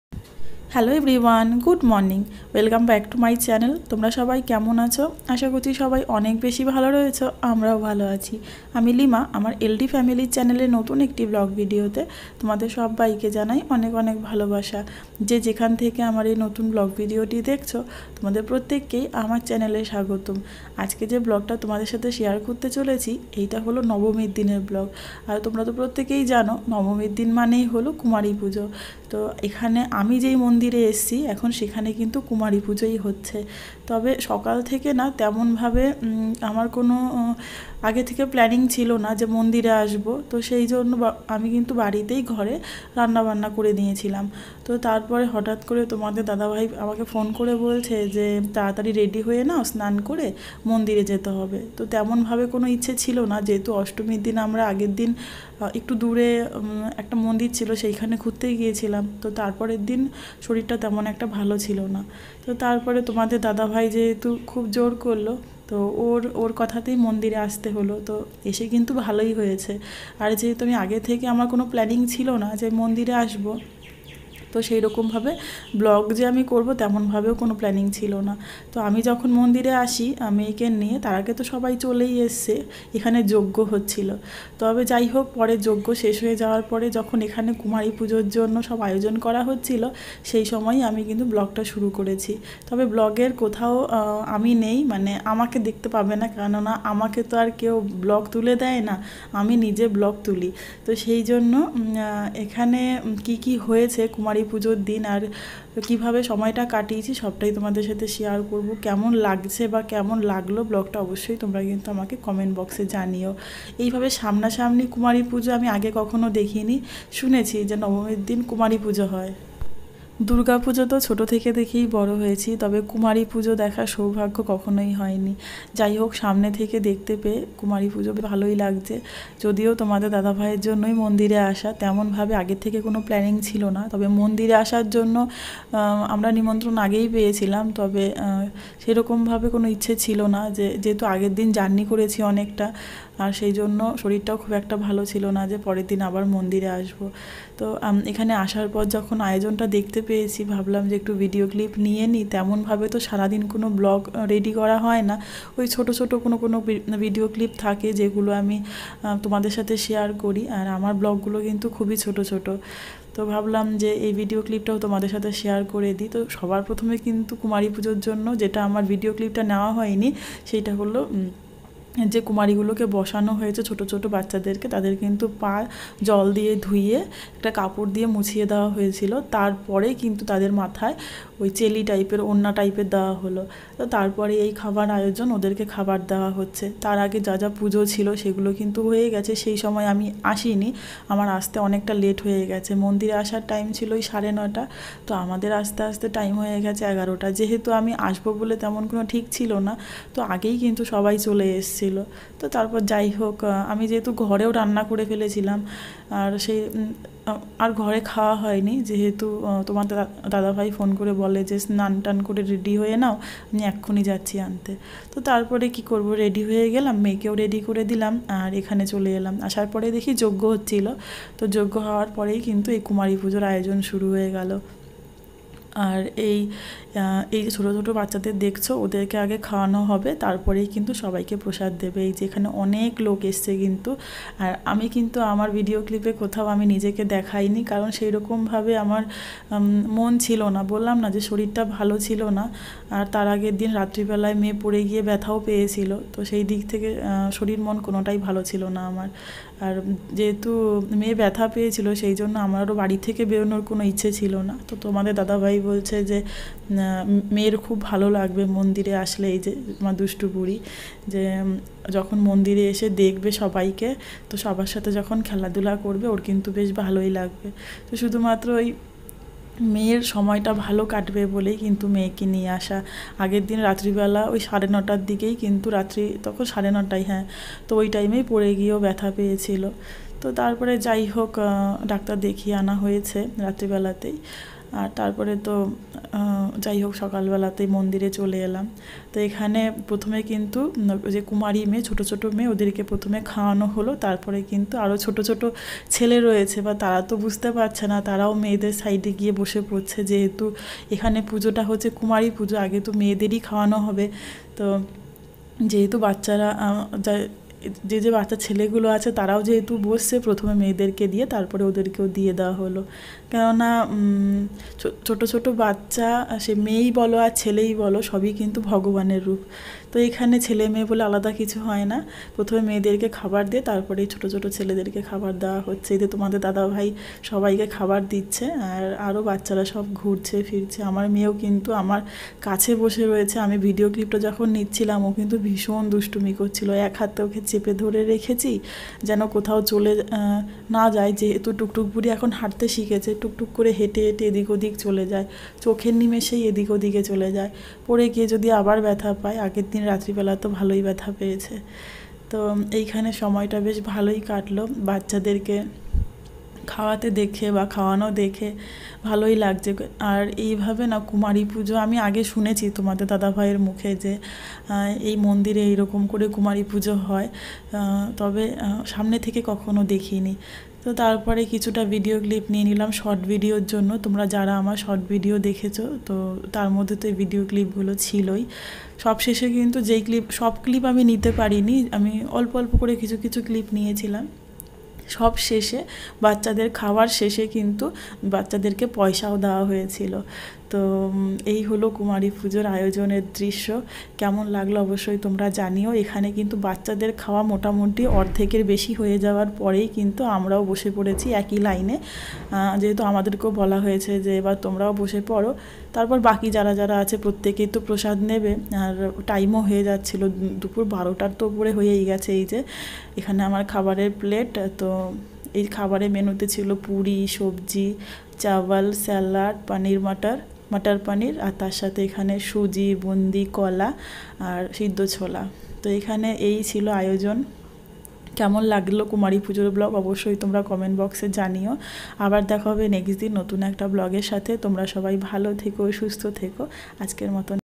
ফৌজি ক্যাম্পে কুমারী পূজার আয়োজন/এই প্রথম সচক্ষে কুমারী পূজা দেখার সৌভাগ্য হয়েছে 🙏
ফৌজি ক্যাম্পে কুমারী পূজার আয়োজন/ এই প্রথম সচক্ষে কুমারী পূজা দেখার সৌভাগ্য হয়েছে 🙏 @LDFamily